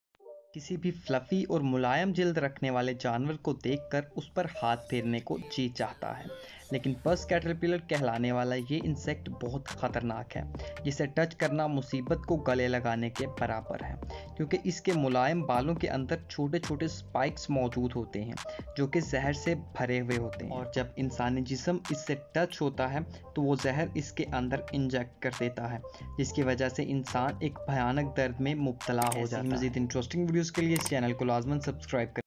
Puss Caterpillar A dangerous Insect
Puss Caterpillar is a smooth good-looking but Dangeorus insect. It has spikes with full of poison.